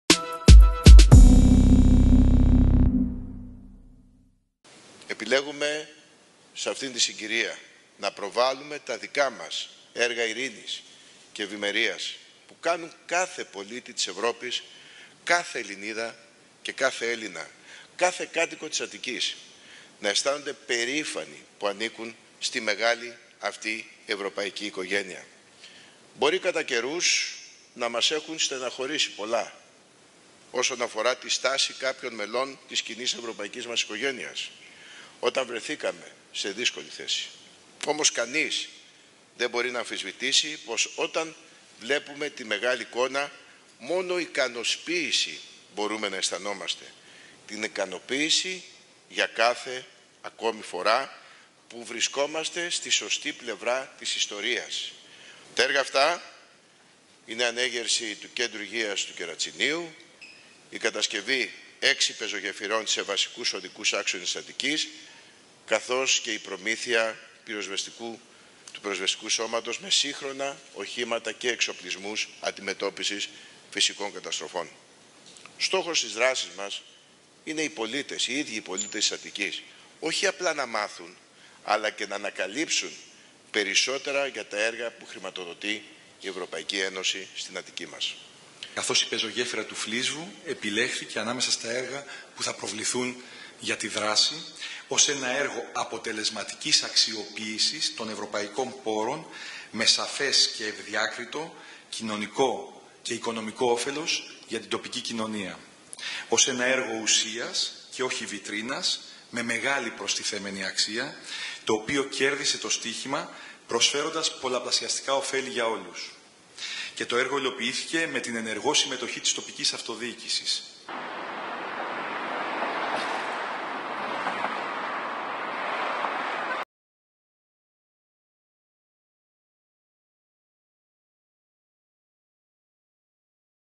Συνέντευξη Τύπου του Γ. Πατούλη για τα έργα που υλοποιούνται με χρηματοδότηση από το ΠΕΠ Αττικής
Στην παρουσίαση εμβληματικών έργων που εκτελούνται στην περιφέρεια Αττικής με χρηματοδότηση από το ΠΕΠ Αττική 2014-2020 και τα οποία έχουν ενταχθεί στην επικοινωνιακή εκστρατεία “ EU In My Region”, («Η Ευρώπη στην περιοχή μου») προχώρησε ο περιφερειάρχης Αττικής Γ. Πατούλης,  στο πλαίσιο συνέντευξης Τύπου στο δημαρχείο Παλαιού Φαλήρου.ΑΠΕ-ΜΠΕ/ΑΠΕ-ΜΠΕ/Κ.ΣΟΡΟΒΟΣ

Γιάννης Φωστηρόπουλος, δήμαρχος Π. Φαλήρου